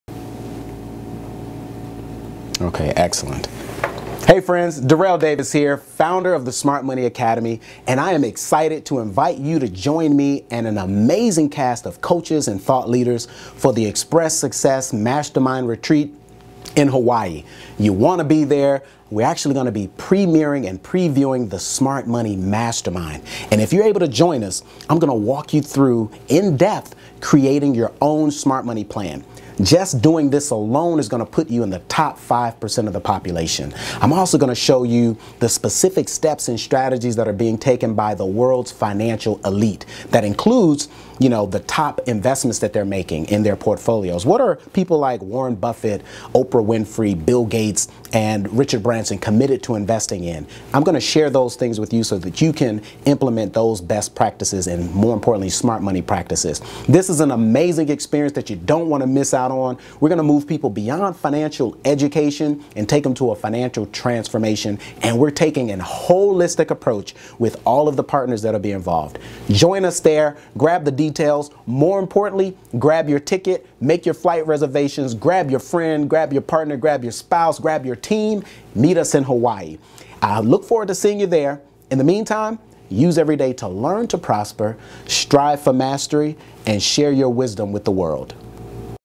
Express Success Smart Money Mastermind Hawaii
Invitation to the Express Success and Smart Money Mastermind Retreat in Hawaii